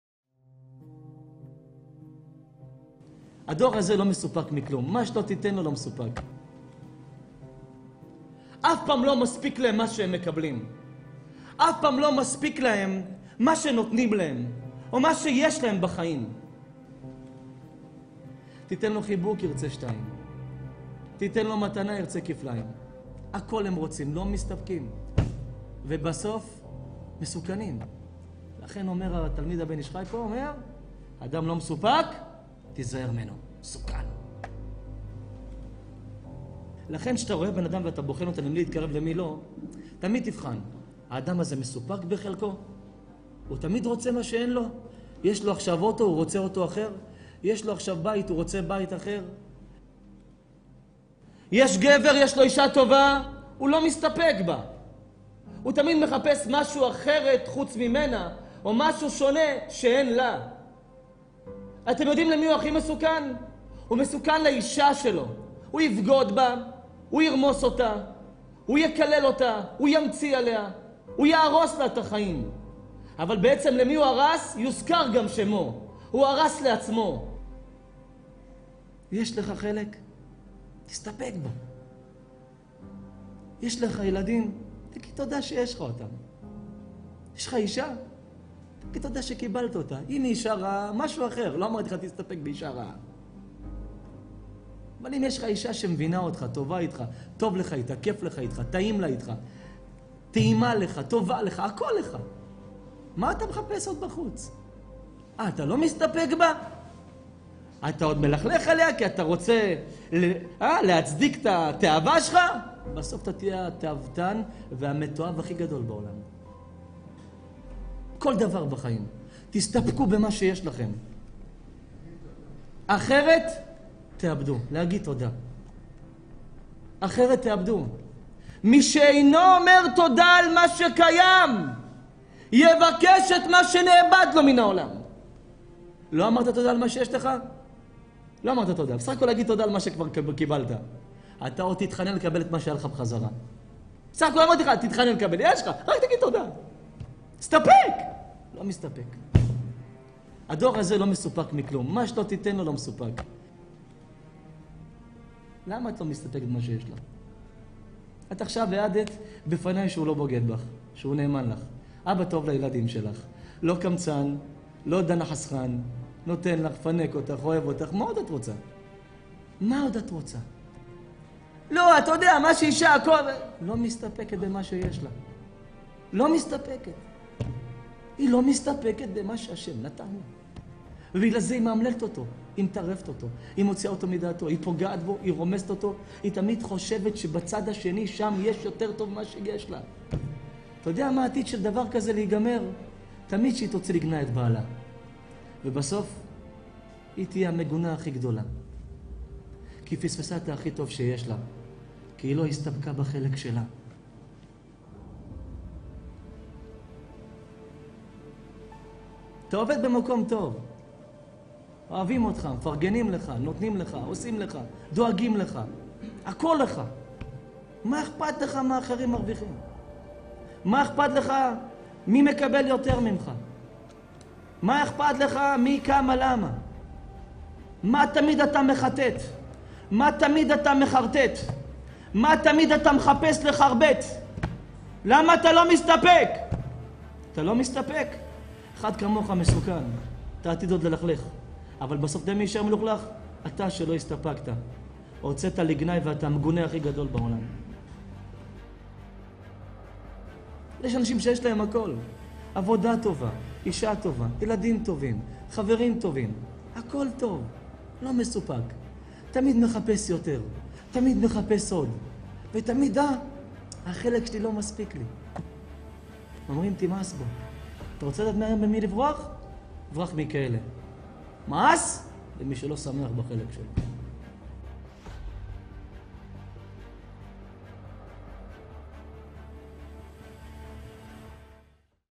הרב רונן שאולוב - מה מפסיד אדם לא מסופק שלא שמח בחלקו ?! מחזק ביותר !!
מהרו להתקין 🤙

שתפו